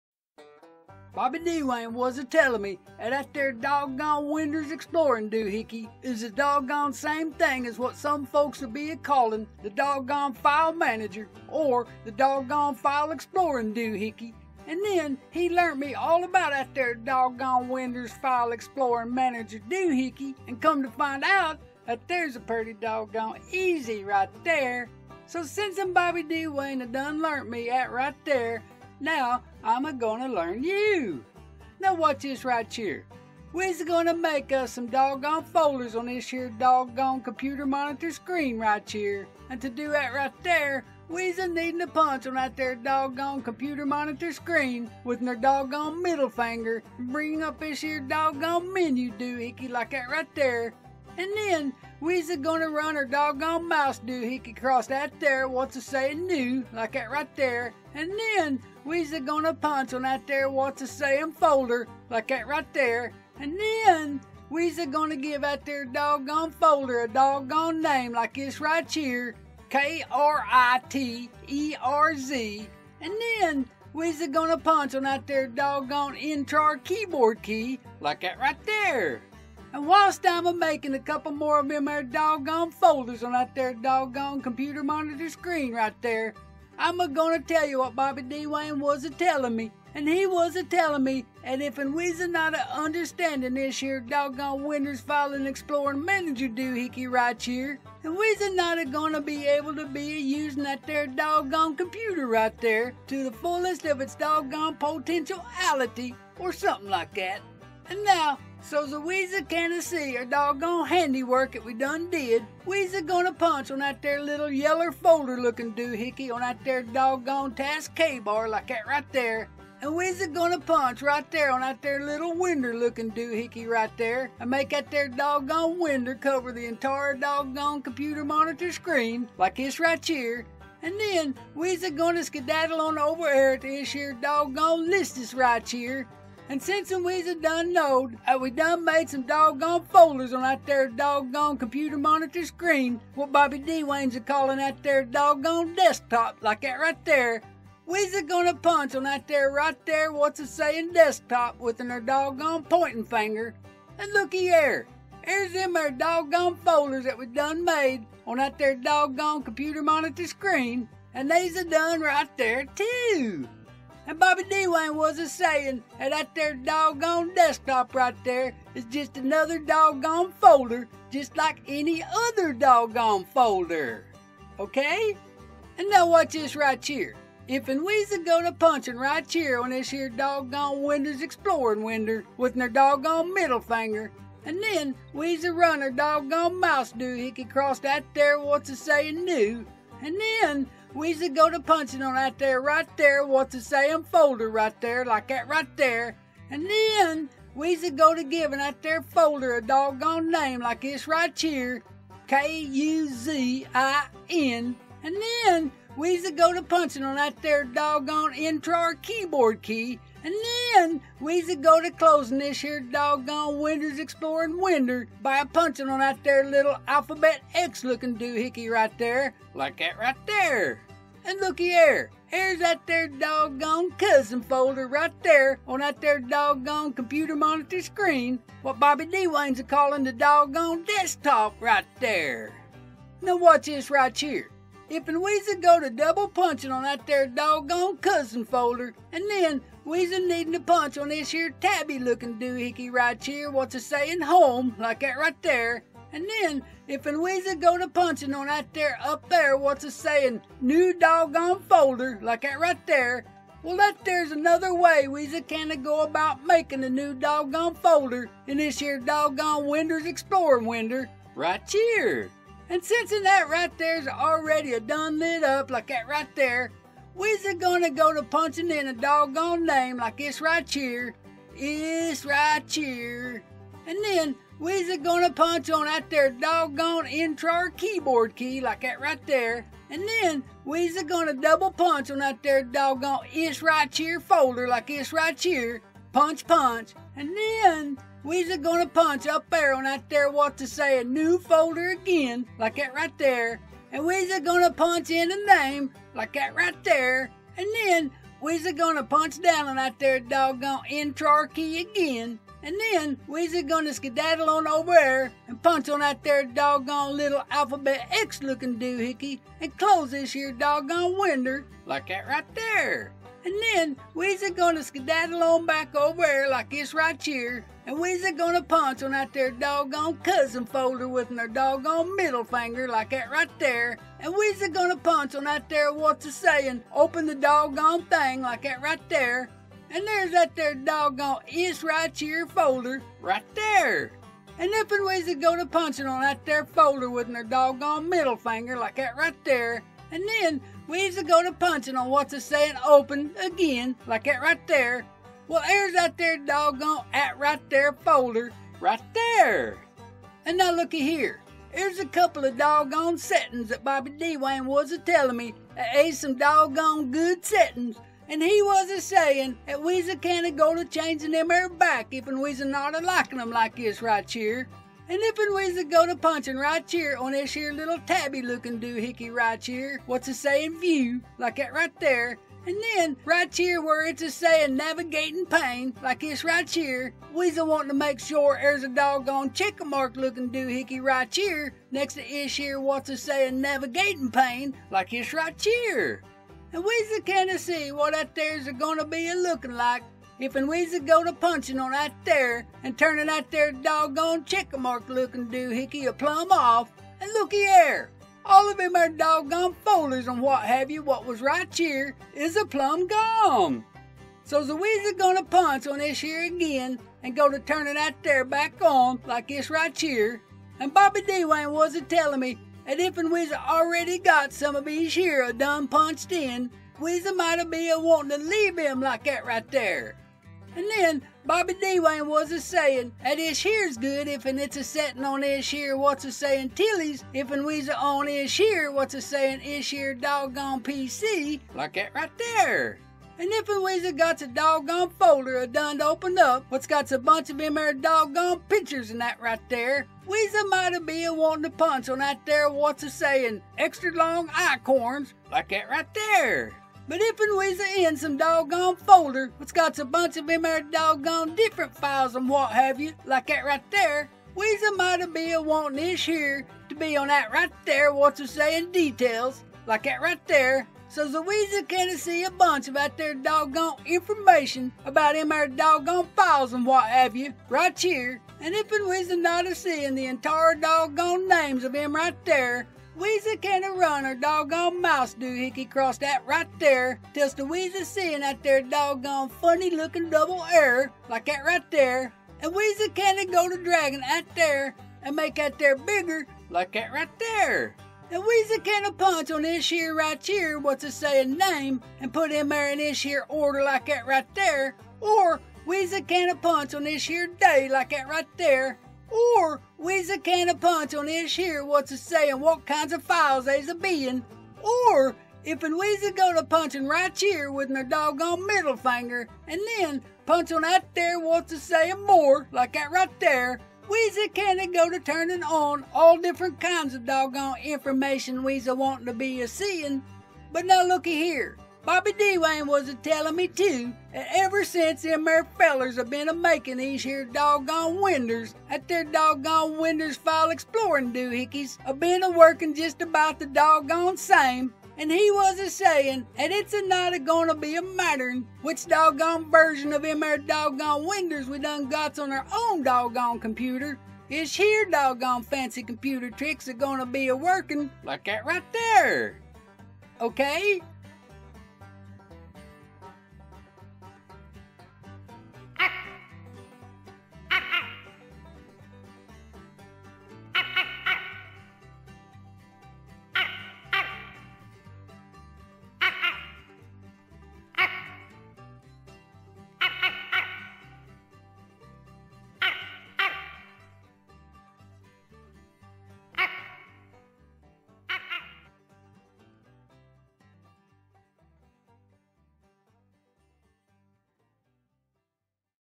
Introduction to File Explorer
My Pops fretted for days when I told him it was time he learned file management. But, I was surprised how quick he learned it.  Now he says ‘i dun mastrd at ritair’